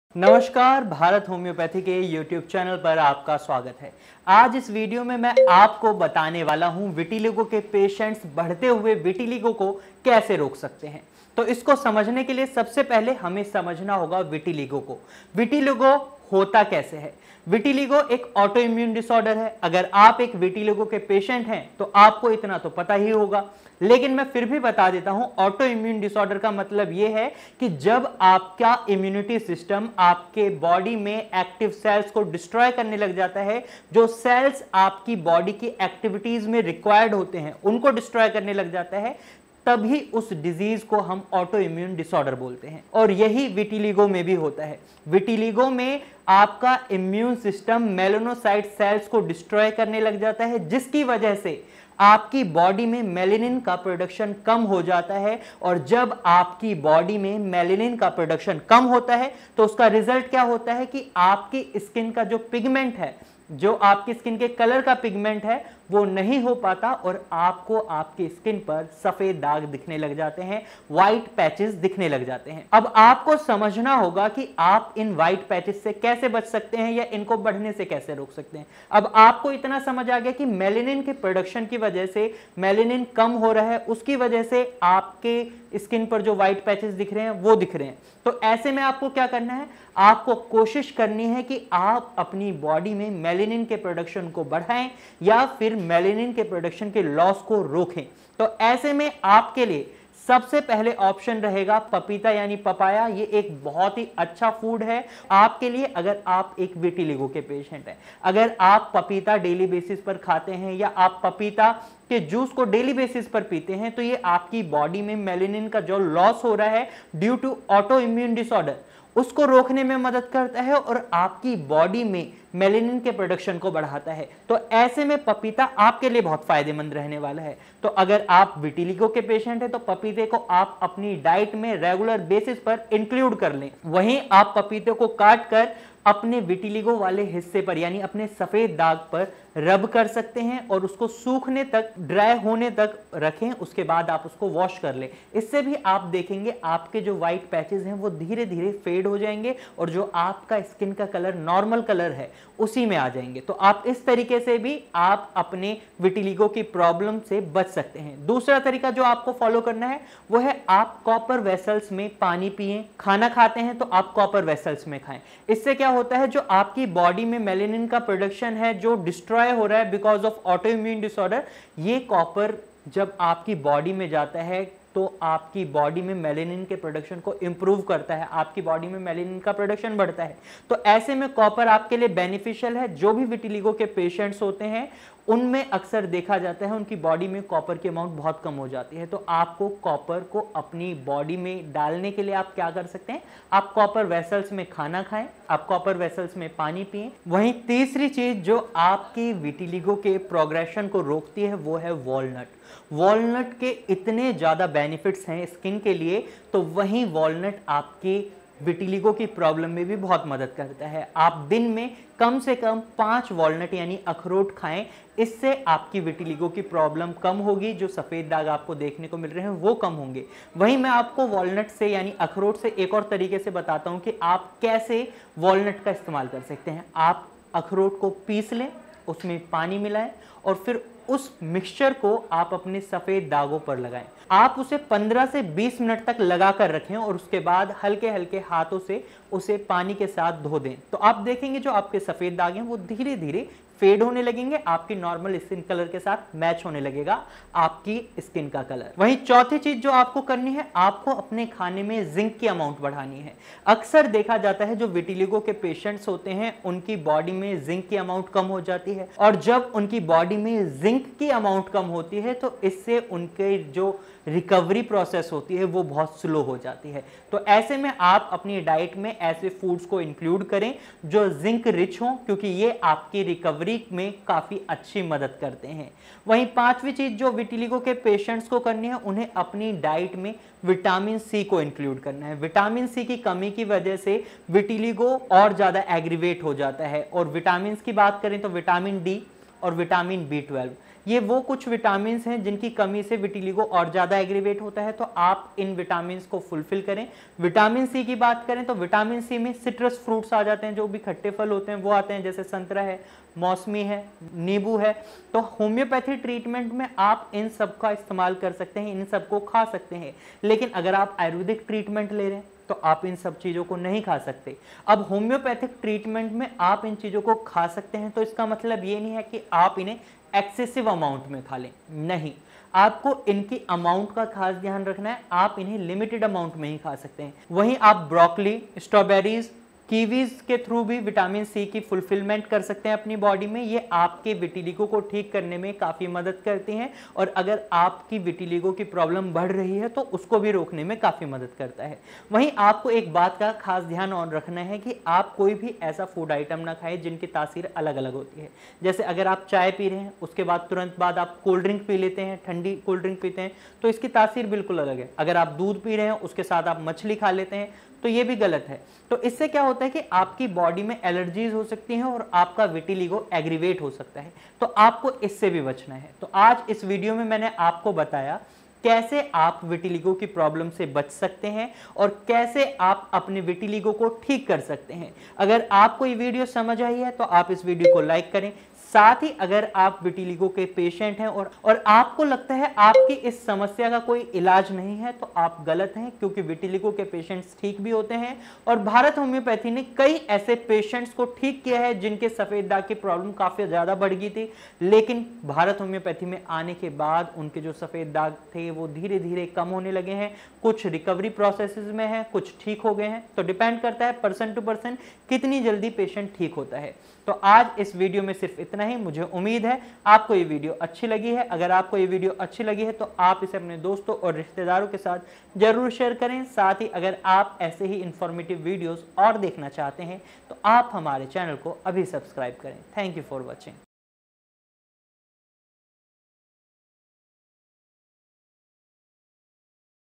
Foods that control spread of Vitiligo | Bharat Homeopathy
Hello, in this video will explain how to control the spread of vitiligo.

Vitiligo is an auto-immune disorder when your immune system starts destroying the active cells in the body. These active cells help in conducting various activities in the body. For example, the patient"s immune system destroys the melanocyte cells in vitiligo.

As a result, it further reduces the production of melanin, which determines skin pigmentation. The lack of melanin causes white spots on the skin.

How can we reduce these white spots?
It would help if you tried to either increase the production of melanin in the body or reduce the loss of
melanin production.

Papaya, It is highly beneficial for white spots patients and should be consumed because it increases the production of melanin in the body.

Use copper utensils- Eat food and drink water in a copper vessel. Copper vessels increase the production of melanin in the body.

Walnut - You should consume at least five walnuts a day. You can grind walnuts and mix the powder
with water. Apply this paste to the white spots and wash it off slowly with water after 15-20 minutes.

Zinc - Foods rich in zinc help a lot in curing white spots.

Vitamins C, D, and B12 - Their deficiency aggravates the condition of vitiligo.

If you are experiencing symptoms, consult Bharat Homeopathy"s expert team of doctors, ask your doubts and start your medication.

Address: A-517, Sushant Lok Phase I, Sector 28 near Iffco chowk metro station, Gurugram -
122009